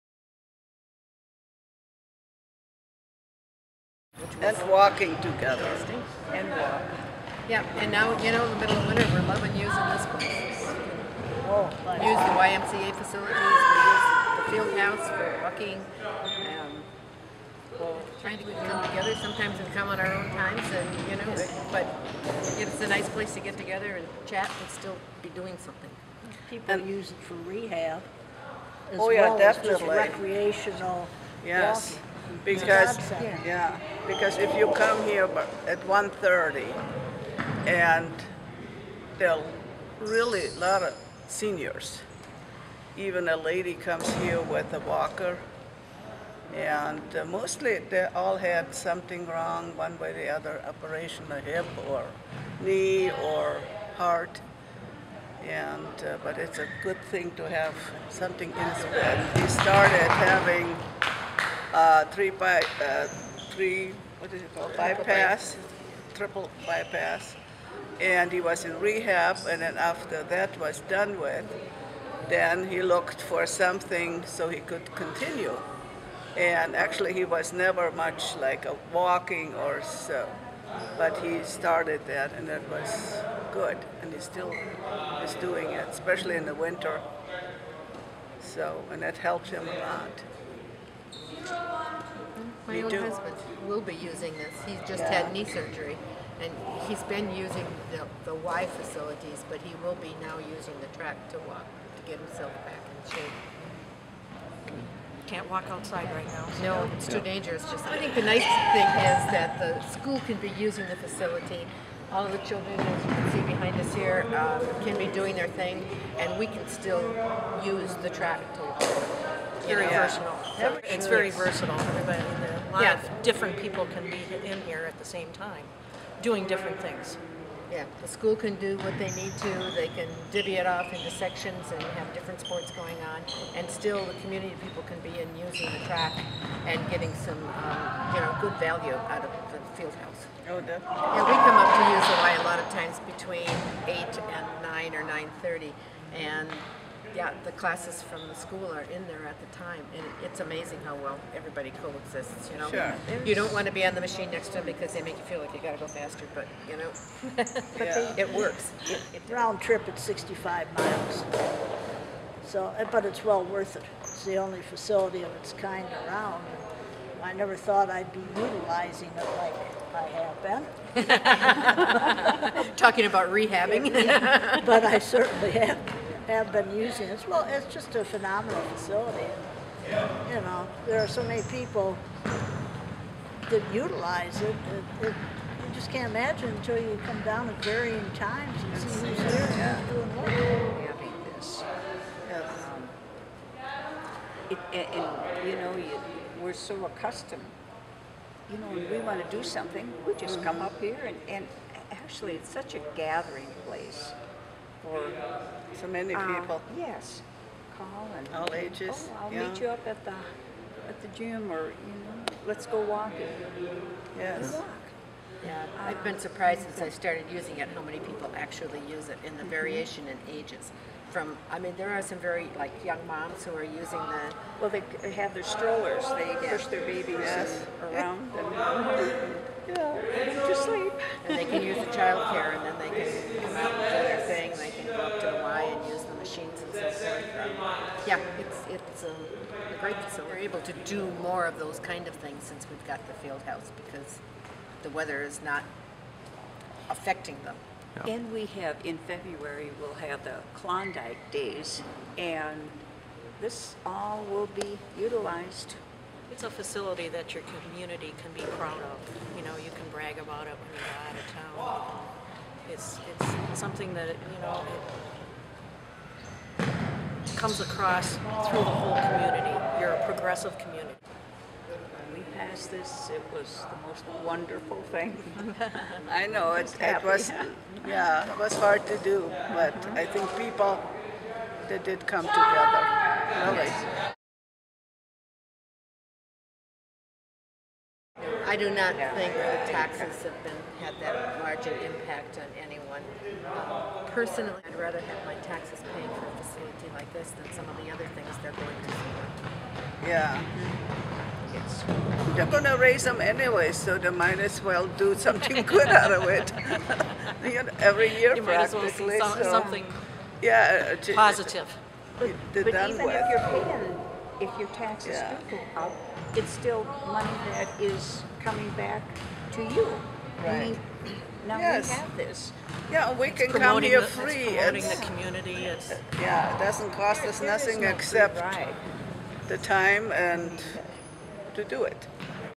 Referendum - Community
Hear how one community benefits from a new community/school facility.